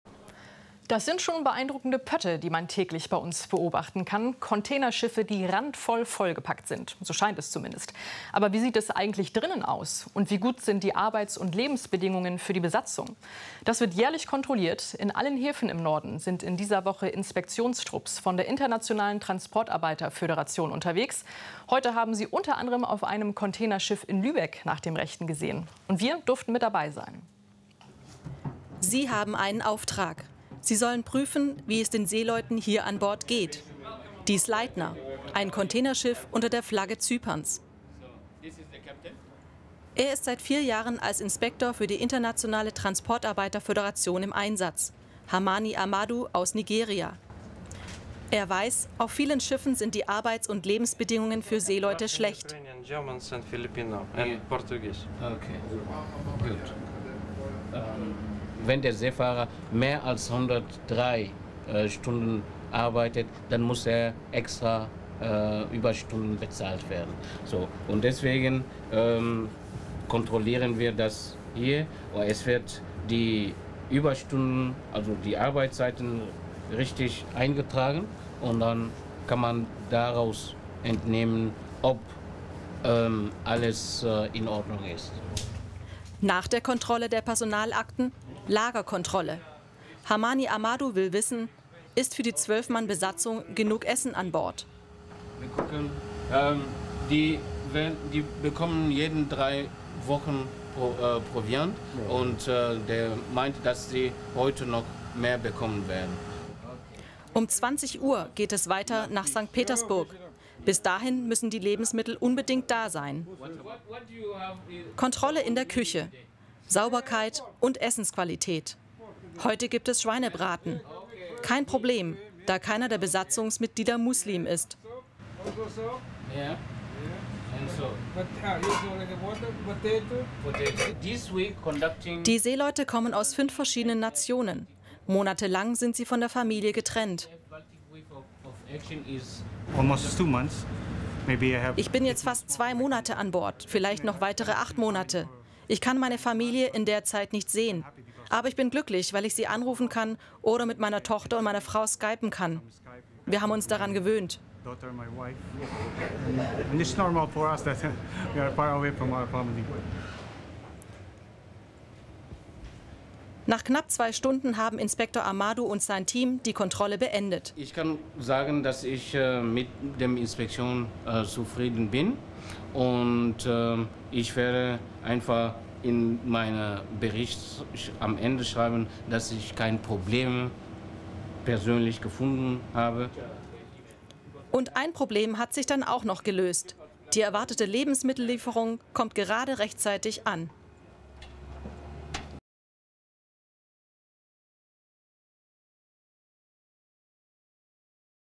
Ein Containerschiff auf dem Prüfstand
Billigflaggenkampagne - Arbeitsbedingungen auf Handelsschiffen auf dem Prüfstand.